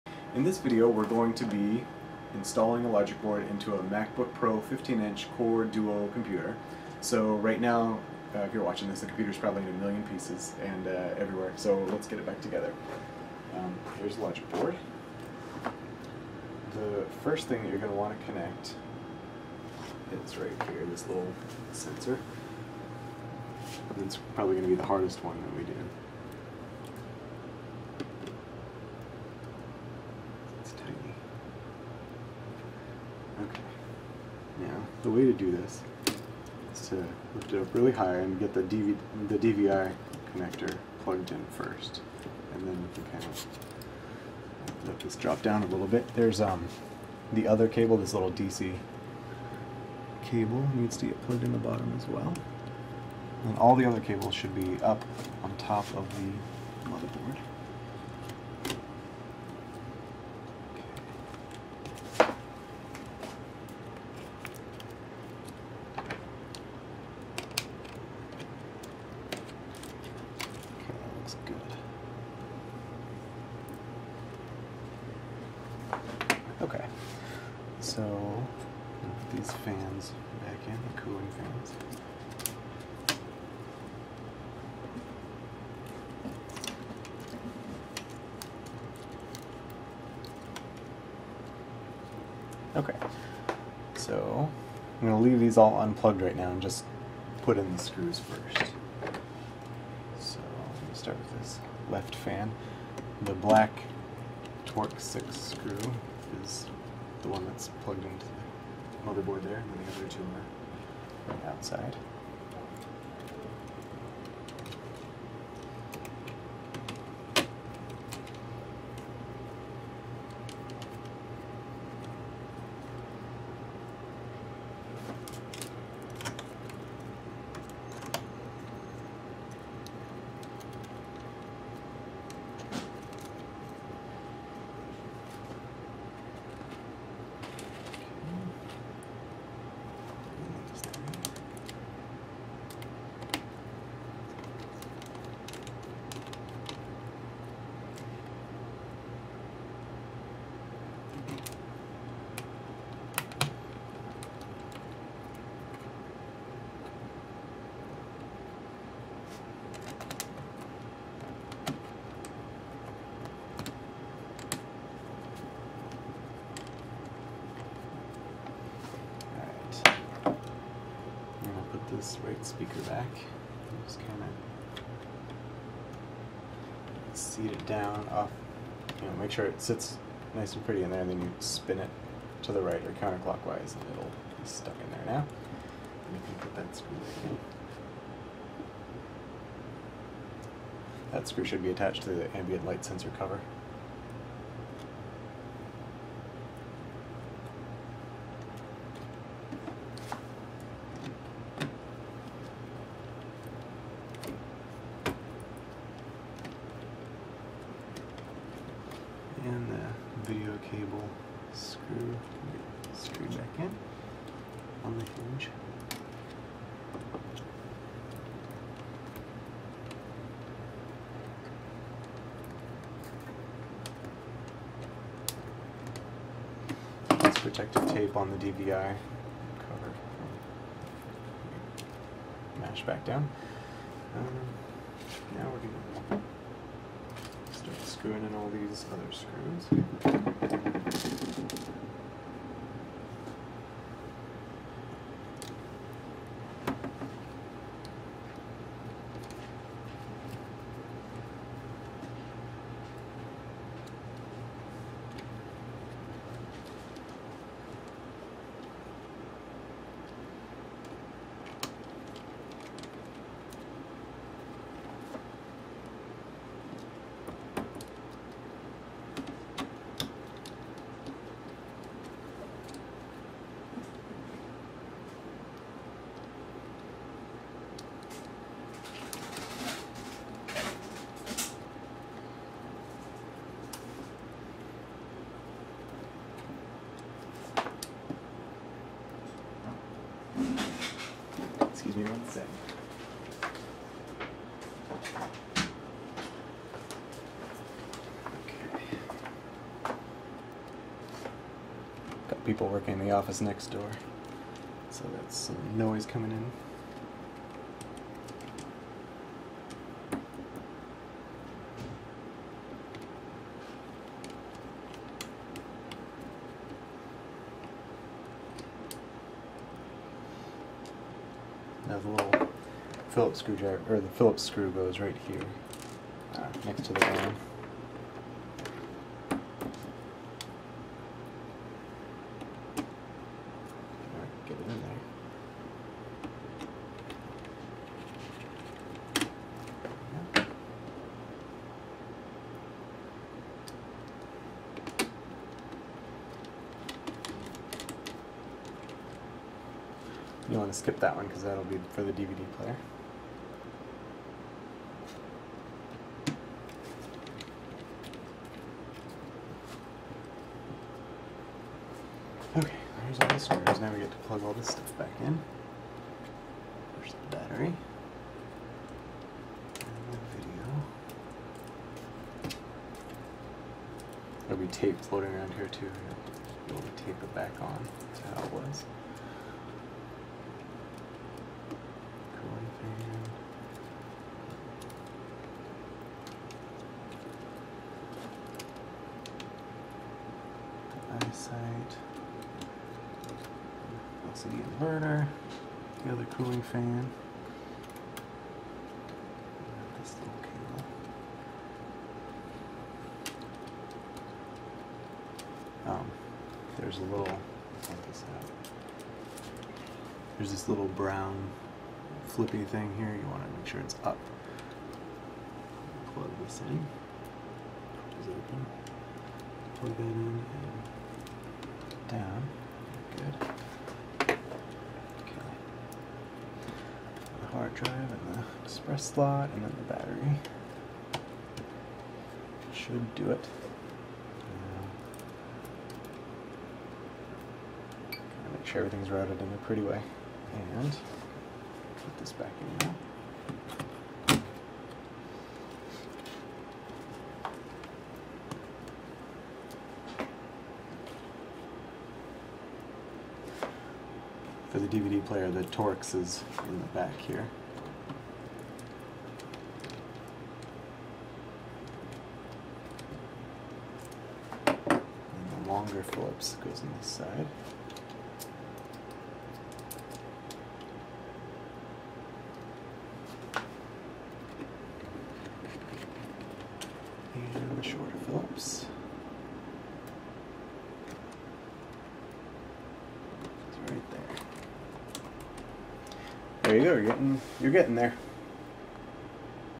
MacBook Pro 15in Core Duo Logic Board and DVD Installation
This video shows you how to install a logic board (along with the cooling fans) and a DVD player into a MacBook Pro 15 inch Core Duo system.    Some of the connectors are hard to see, but you can get a pretty clear idea where they plug in from the location of the connector on the motherboard and the location of the connector.  Hit me back if you have troubles, or check out for pictures.